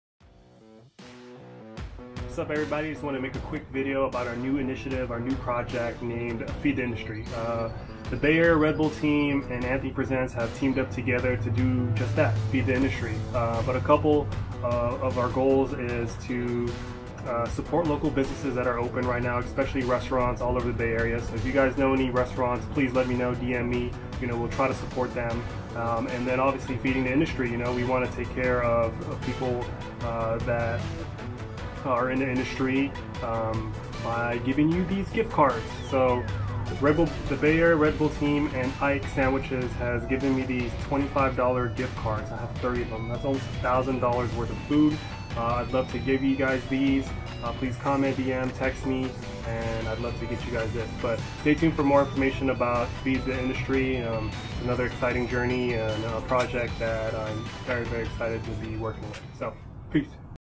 FEED THE INDUSTRY: Introduction
Brought to you by the Bay Area Redbull team (Amy, Jameson, and Matt) and Anthony Presents, Feed The Industry is a unique initiative to support local businesses and provide meals to workers in the industry.